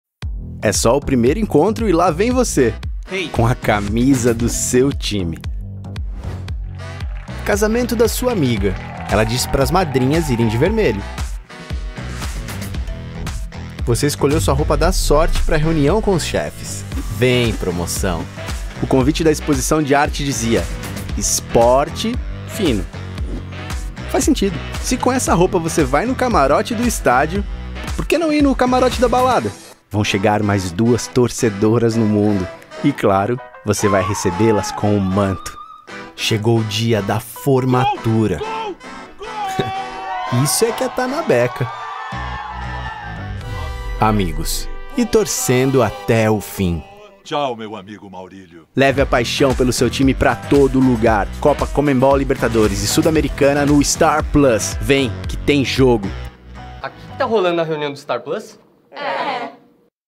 Camisa de time é para ser usada em qualquer lugar, sim. VEM QUE TEM JOGO! | Star+
Primeiro encontro, casamento e até na hora do parto: leve a paixão pelo seu time para todo lugar. Vista o manto sagrado quando e onde quiser. O Star+ te entende e incentiva. VEM QUE TEM JOGO!

Assista à Conmebol Libertadores e Sudamericana no Star+.

Clique em "Inscrever-se" para conhecer todas as novidades do Star+ antes de todo mundo!